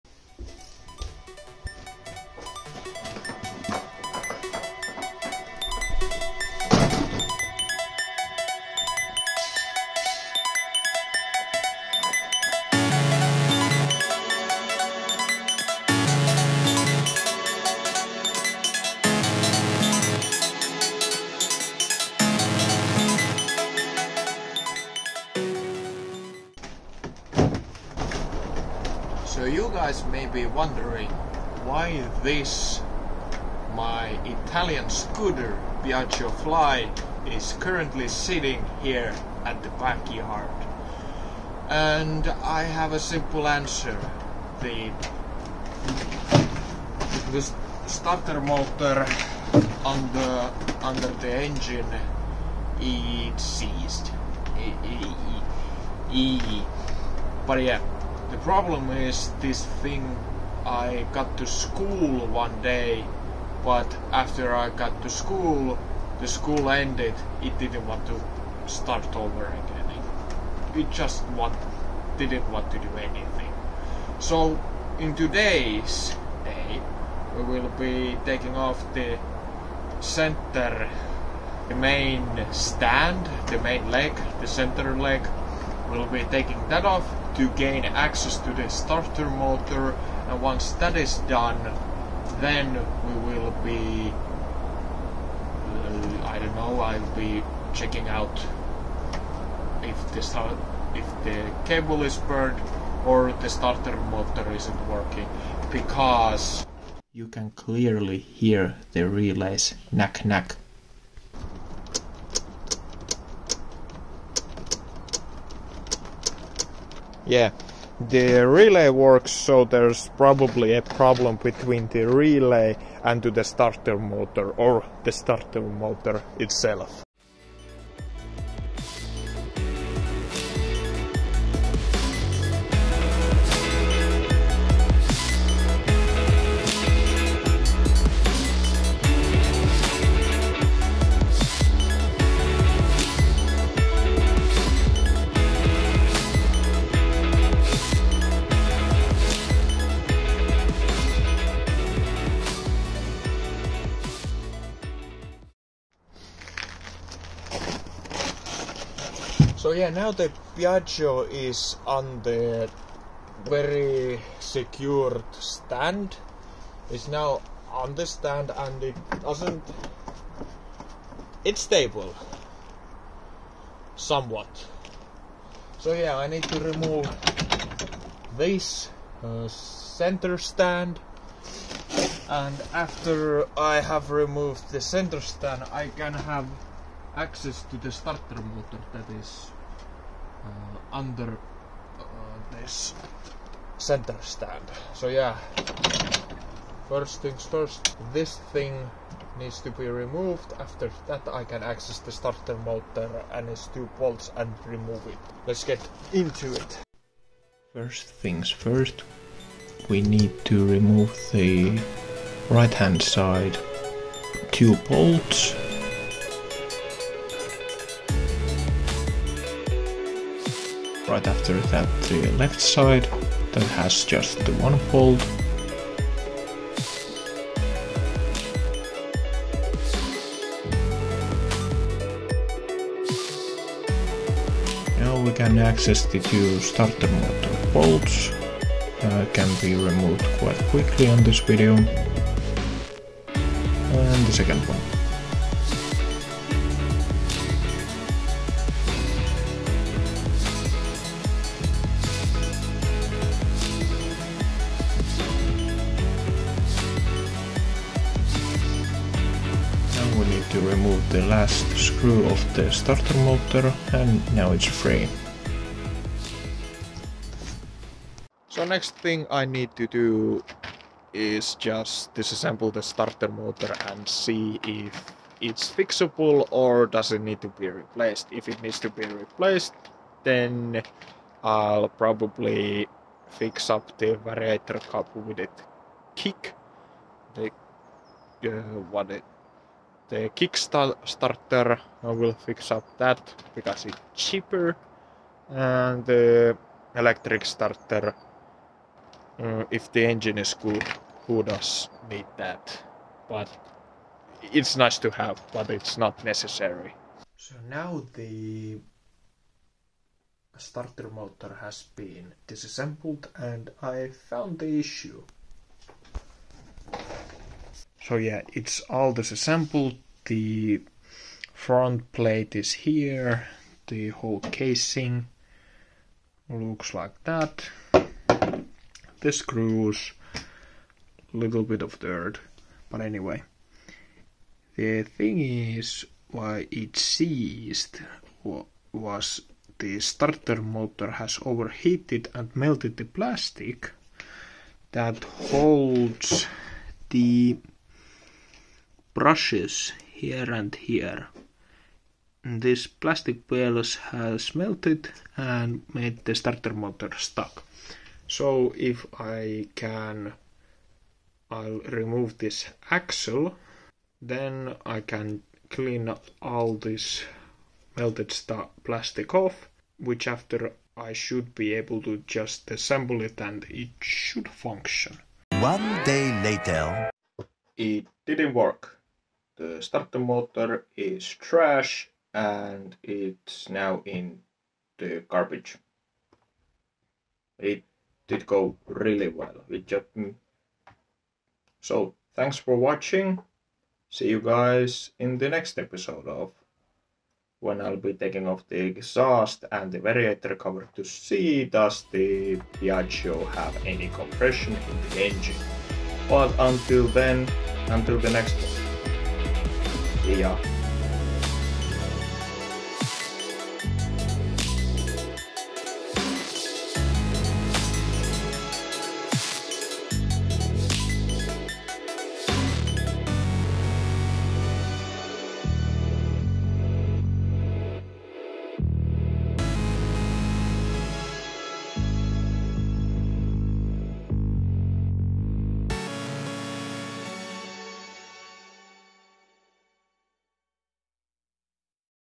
Scooter On A Stand. (RED Piaggio)
Be honest, when the last time you seen Italian scooter? Not like yesterday as I have. Cause they are always broken. Just joking, they are great when they haven't been tinkered with.